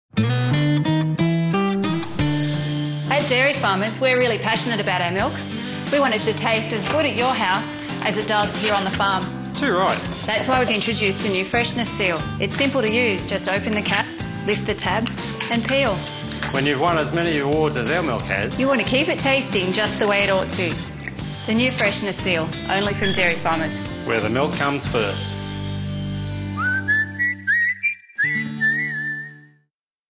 Dairy Farmers' Milk with Freshness Seal
Dairy Farmers in Australia introduces their milk with an induction heat seal to preserve freshness.